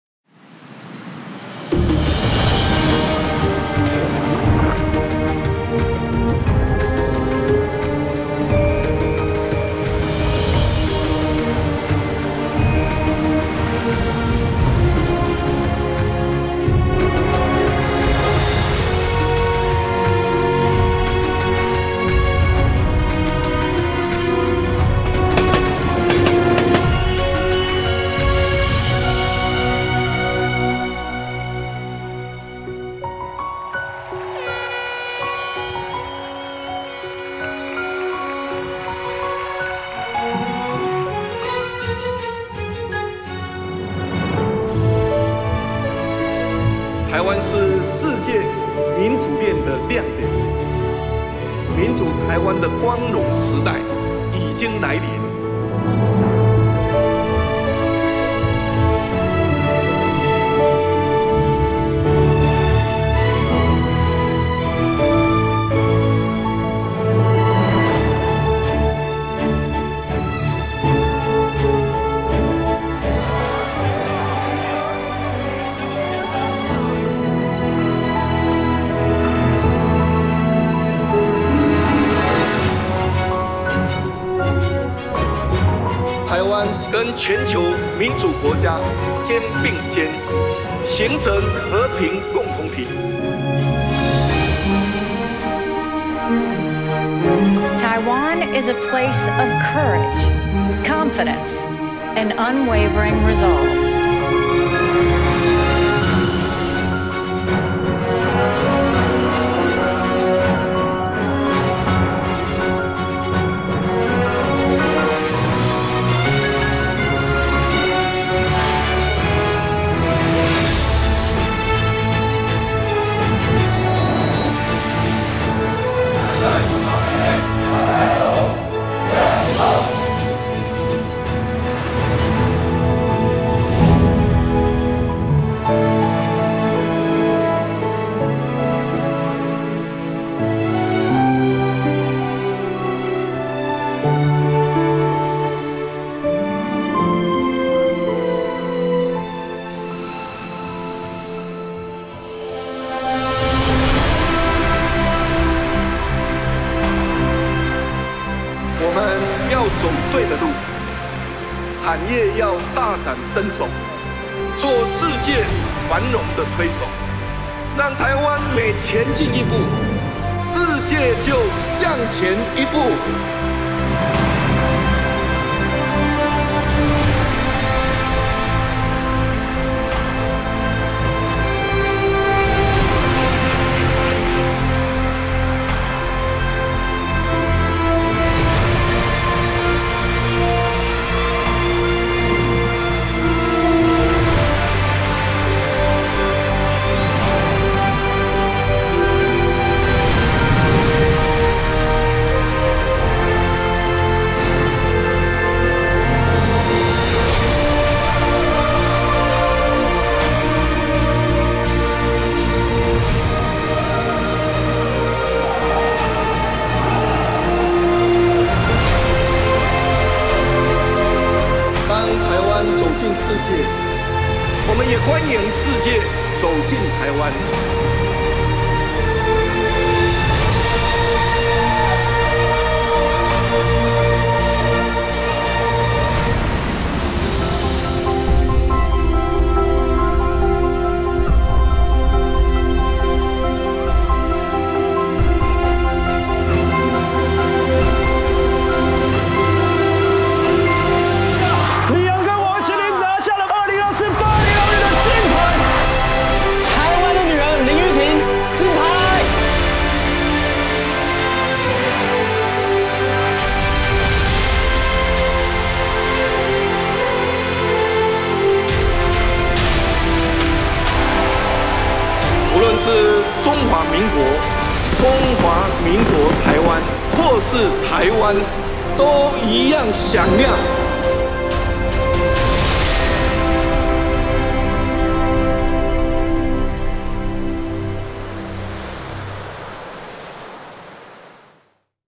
A democratic, peaceful, and prosperous new Taiwan - 2024雙十國慶影片「民主和平繁榮的新台灣」
為慶祝2024雙十國慶，外交部特別製作「民主和平繁榮的新台灣」國慶影音短片，內容包括「民主」、「和平」、「繁榮」及「台灣新世界、世界新台灣」四個篇章，分別強調台灣作為民主世界之光、世界和平舵手及全球繁榮推手的重要性，並展現台灣自信勇敢地走向世界，也歡迎世界走進台灣的胸懷。

感謝授權名單：

總統府
行政院
立法院
司法院
文化部
中央選舉委員會
經濟部產業發展署
經濟部國際貿易署
交通部觀光署
內政部國家公園署玉山國家公園管理處
交通部觀光署北海岸及觀音山國家風景區管理處
交通部觀光署雲嘉南濱海國家風景區管理處
交通部觀光署澎湖國家風景區管理處
交通部觀光署馬祖國家風景區管理處
國防部軍事新聞通訊社
宜蘭縣政府
屏東縣政府
臺北市政府體育局
臺北市政府觀光傳播局
新北市政府體育局/110年全國運動會
新北市政府觀光旅遊局
臺南市政府體育局
臺南市政府觀光旅遊局
嘉義縣政府/亞洲無人機AI創新應用研發中心
臺東縣蘭嶼鄉公所
中華文化總會
中華民國羽球協會
中華民國對外貿易發展協會
中華奧林匹克委員會
台灣電力公司
桃園國際機場股份有限公司
財團法人台灣設計研究院
財團法人國際合作發展基金會
國立海洋生物博物館
「海│李炳曄科技藝術實驗室，國立臺灣美術館授權提供，影片製作：泰墨創藝影像有限公司」
國家中山科學研究院
國家太空中心
國立臺灣大學機械工程學系
中華職業棒球大聯盟
台灣帆船協會
佛光山佛陀紀念館
ELTA愛爾達電視
Newtalk News
TVBS NEWS
三立LIVE新聞
中天電視
中視新聞
公視新聞網
台北101
台視新聞 TTV NEWS
民視新聞網 Formosa TV News network
年代新聞CH50
東森新聞 CH51
客家新聞 Hakka News
原視新聞網 TITV News
華視新聞 CH52
壹電視NEXT TV
寰宇新聞 頻道
鏡新聞
大都會汽車客運股份有限公司
台灣高速鐵路股份有限公司
桂盟企業股份有限公司
誠品書店
璟鋒工業股份有限公司
林建享導演
脫北馬丁
黃慶家